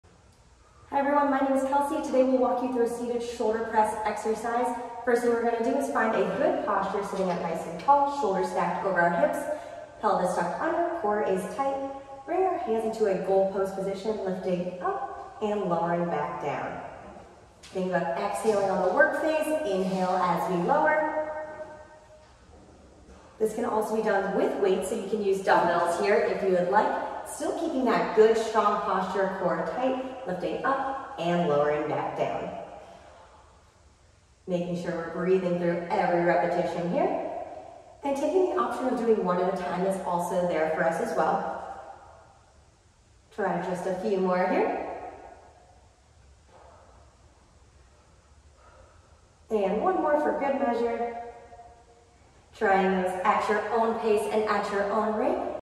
Physical Health and Wellness: Seated Shoulder Press
Our Physical Health and Wellness team is working hard to help you with your exercise routine.  Watch as our Physical Health and Wellness teammate runs through the seated shoulder press exercise.